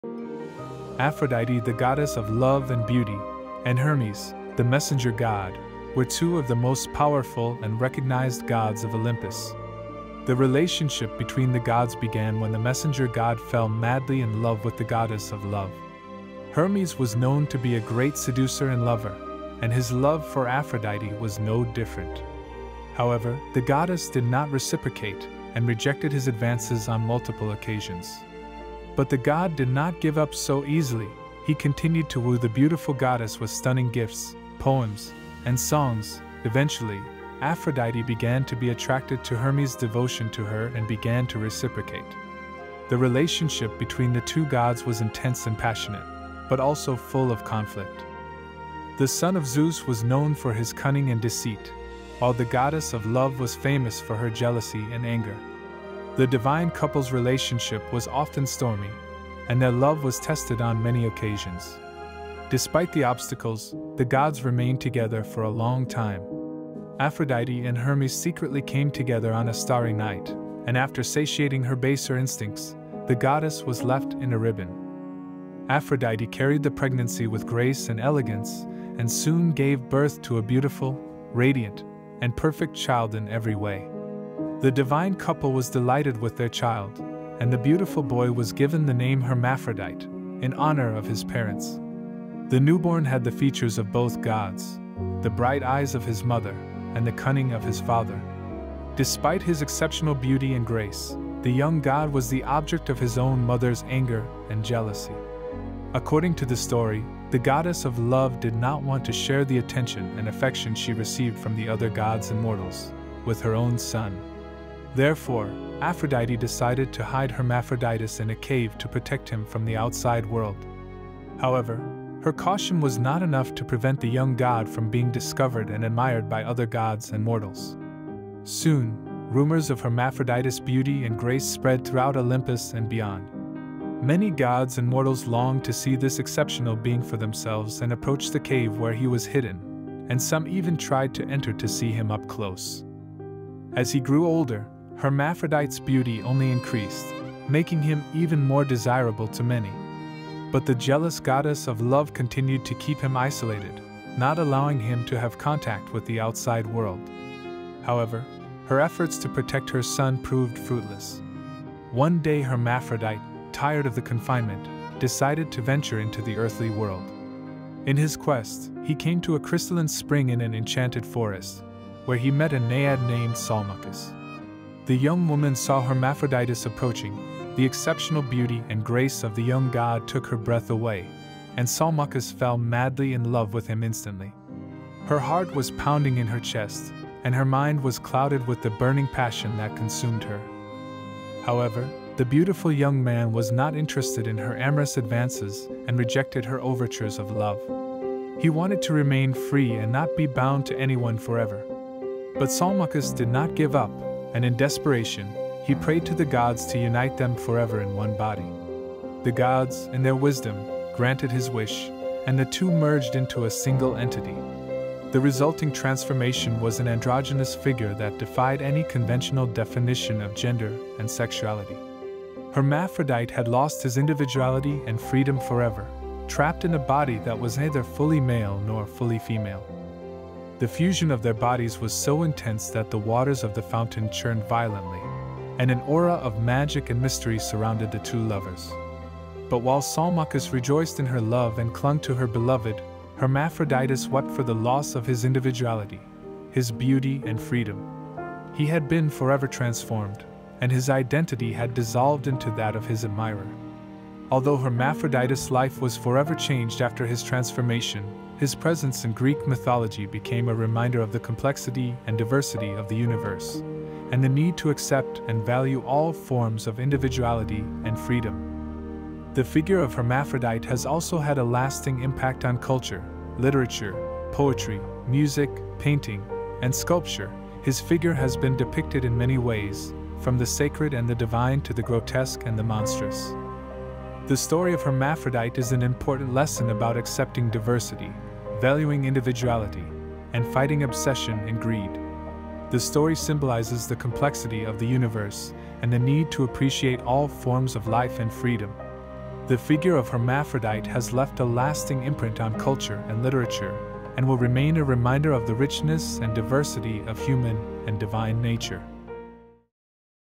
Hermaphroditus - The Myth of the Son of Hermes and Aphrodite - Greek Mythology
Discover the intriguing story of Hermaphroditus, the child of Hermes and Aphrodite in Greek mythology. In this video, we'll explore the origins and adventures of this unique character who possessed both male and female traits, and how their myth has left a profound mark on culture throughout the centuries. Join us on this exciting journey through the world of Greek mythology and learn about the life and legacy of Hermaphroditus.